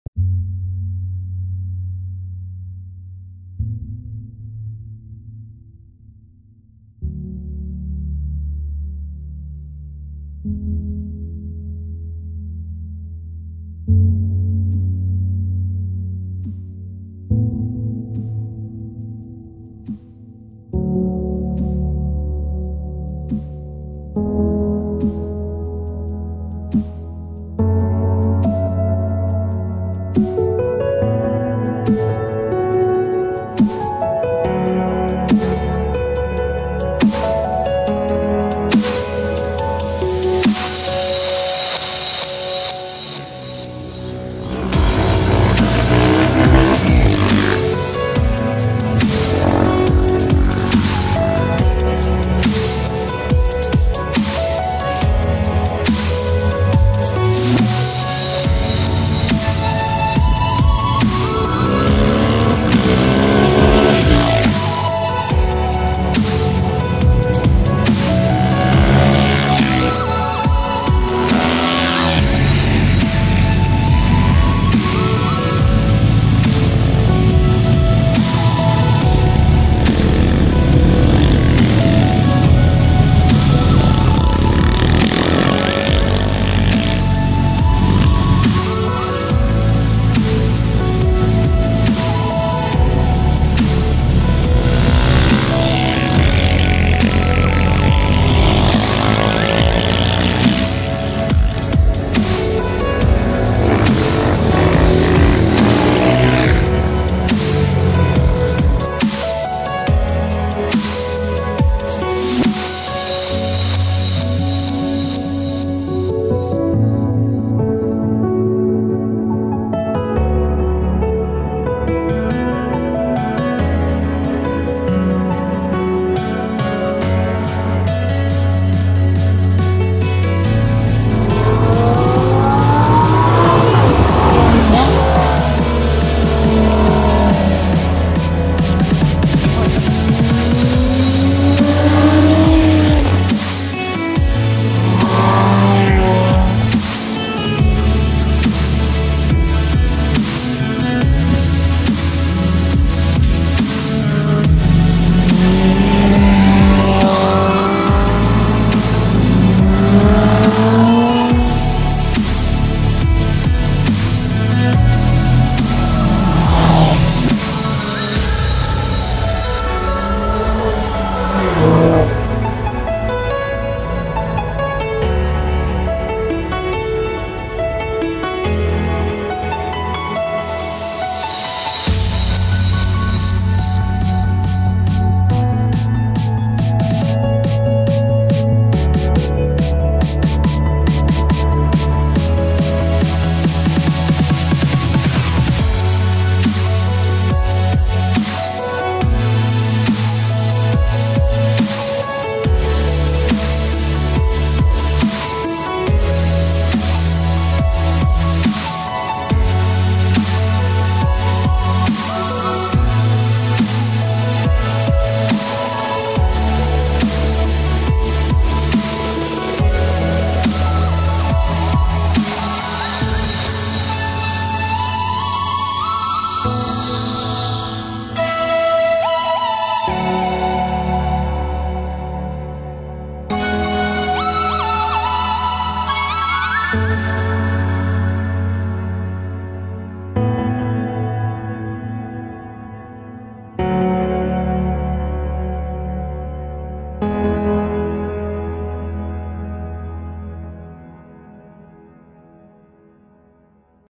Northern Lotus Group | DATO SAGOR CARS & BIKES TRACKDAY 2020
8/3/2020 from Penang to Dato Sagor, 2 hours drive to reach here.
Thanks to those who made it happen.

Shot on samsung galaxy S10+ with DJI osmo mobile 2